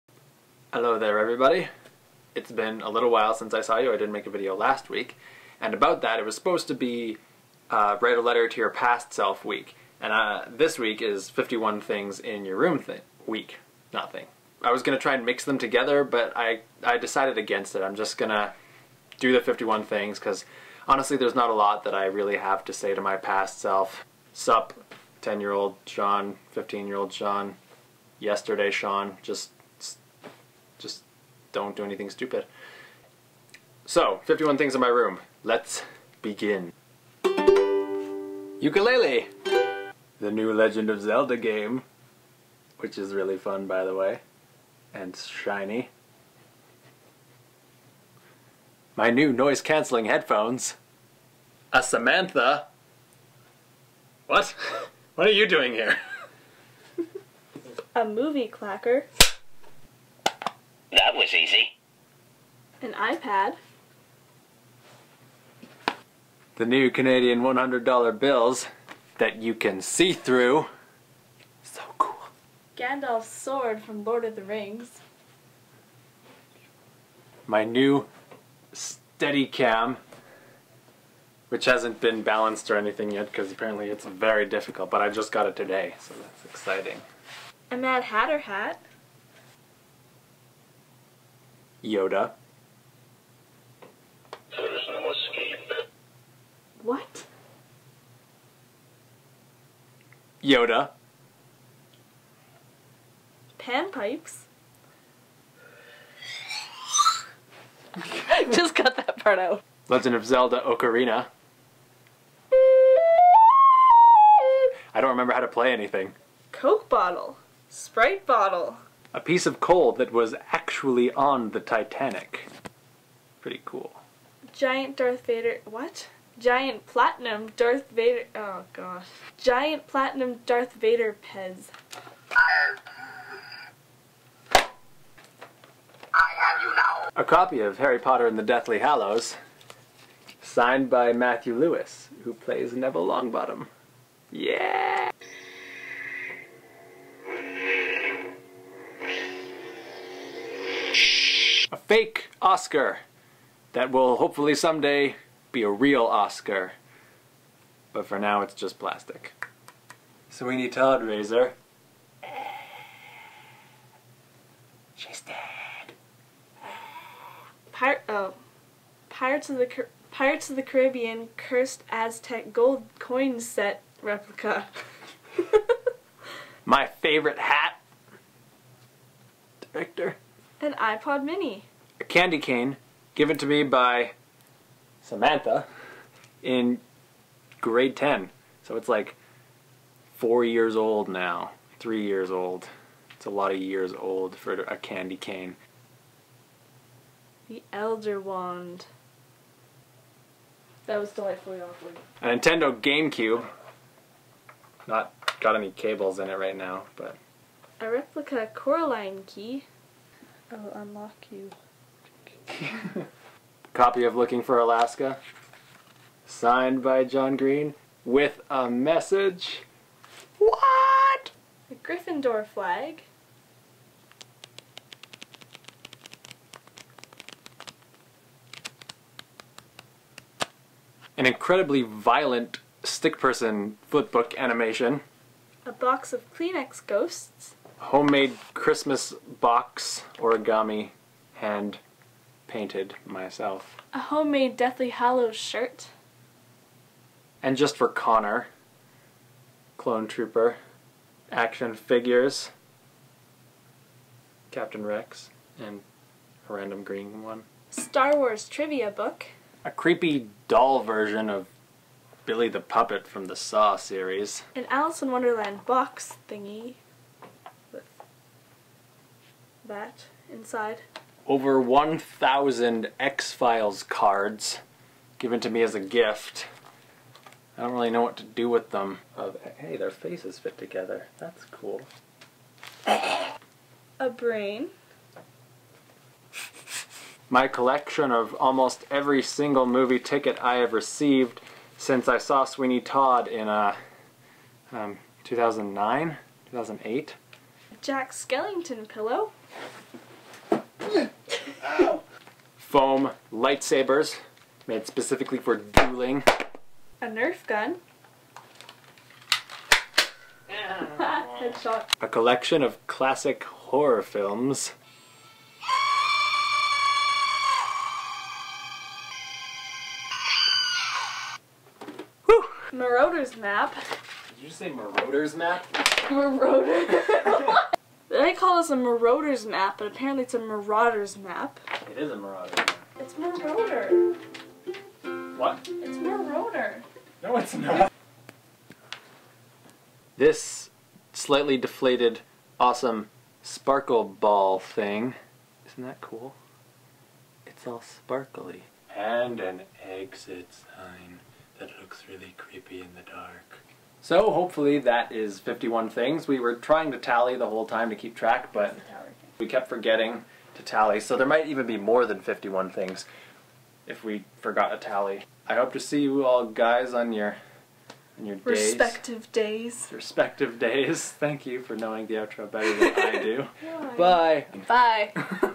December 11th: 51 things
In which Shawn (and Samantha) showcase 51 wonderful items found in his room.